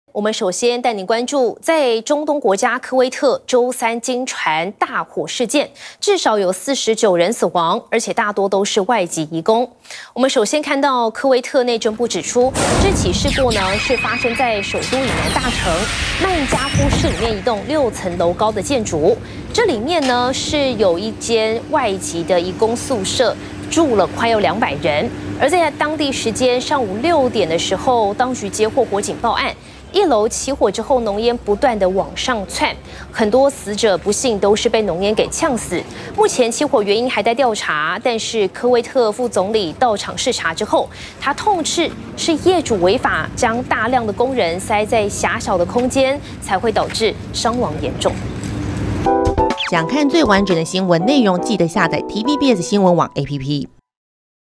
科威特驚傳移工宿舍大火 49人遭濃煙嗆死｜TVBS新聞 20240601@TVBSNEWS02
中東國家科威特週三驚傳發生大火，至少49人死亡，多為外籍移工。